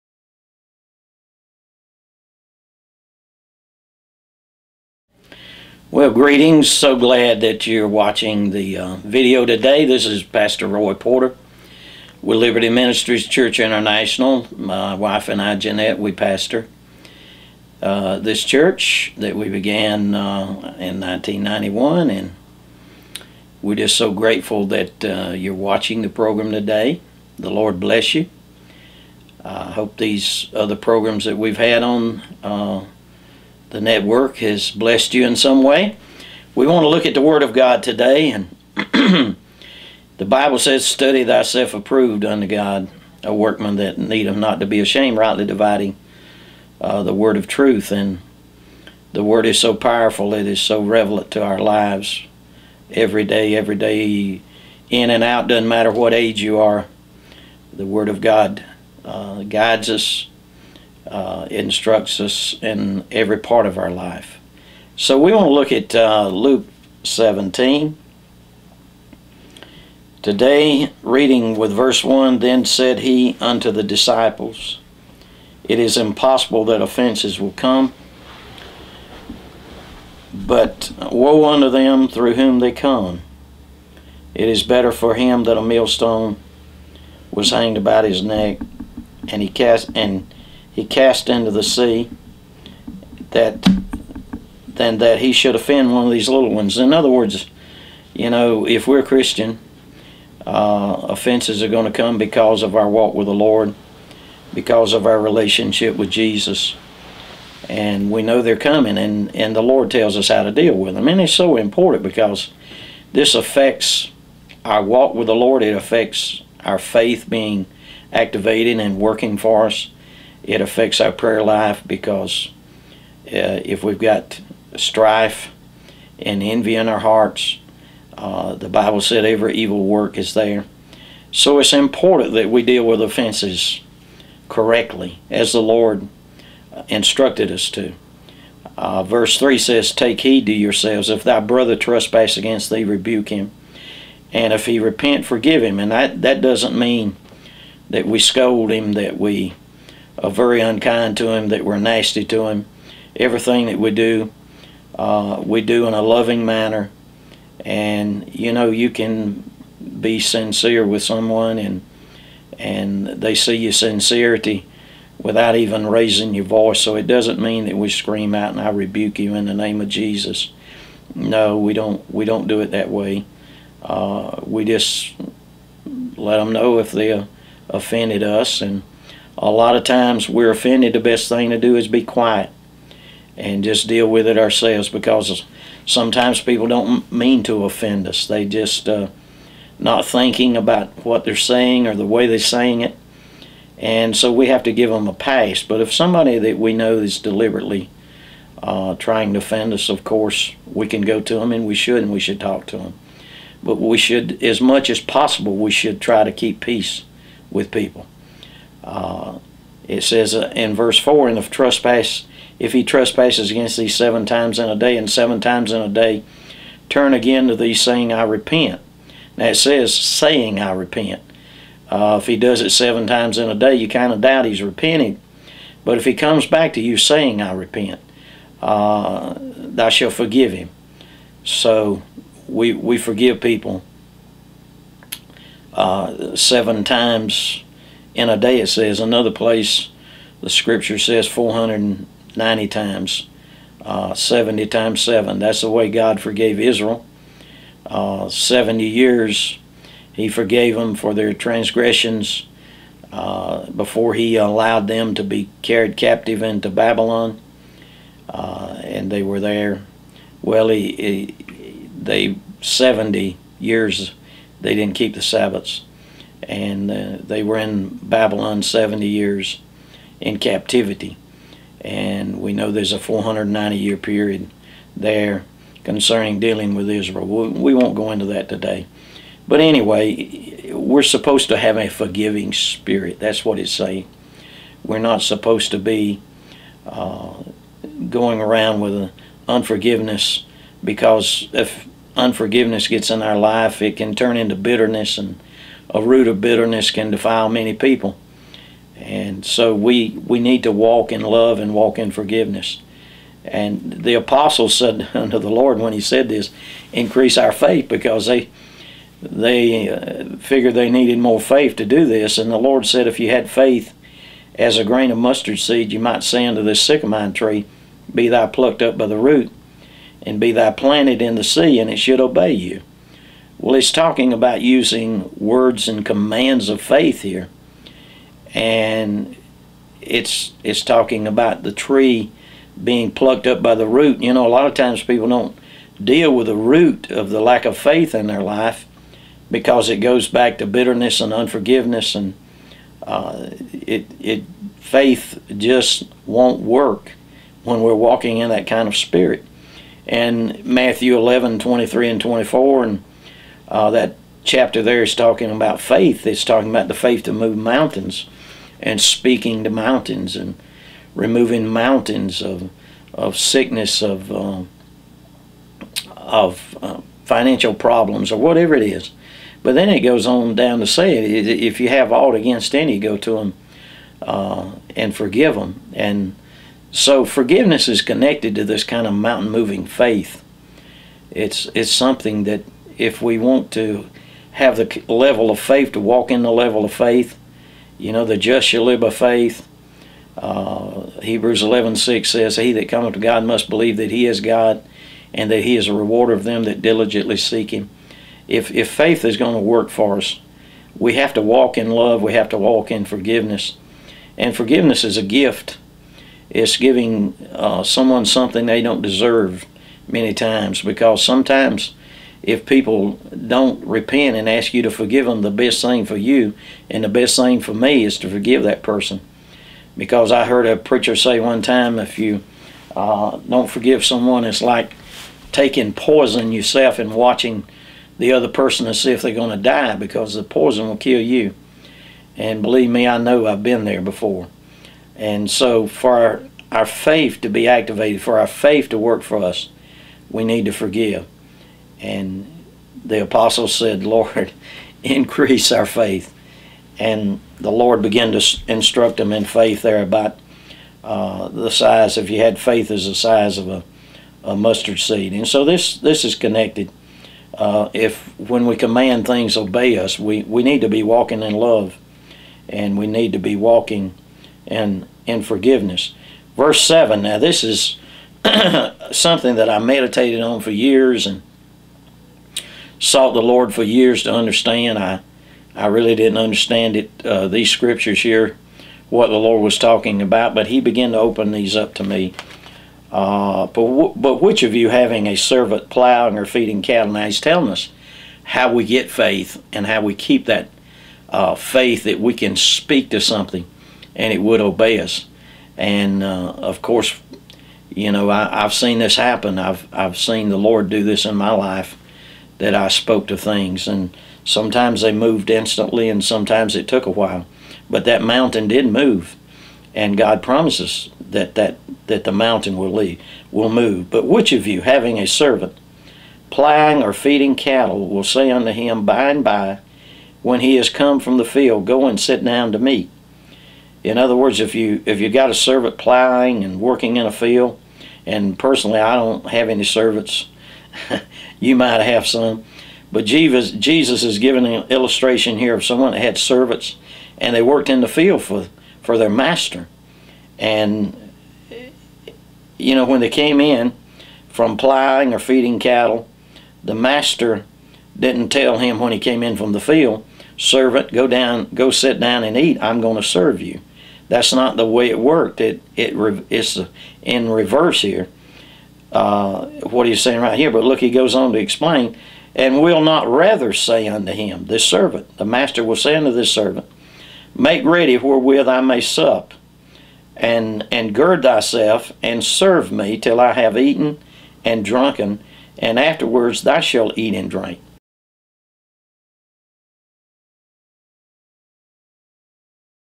Feed the Father Before the Children - Part 1 - Liberty Ministries - Roy Porter
A kingdom principle taught by Roy Porter from Liberty Ministries on the order in the kingdom of God. Feed the Father first then what you need will come.